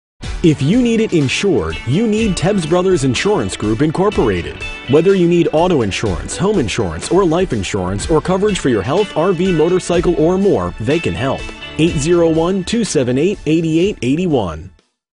Tebbs Brothers Insurance Group, Inc - Salt Lake City, UT
Tebbs Brothers Insurance Group, Inc., is a family owned and operated business and we are committed to providing all of our customers with friendly, reliable service. Our dedicated team provides insurance coverage for individuals, families, and businesses throughout our area for the lowest rates available. for a free quote.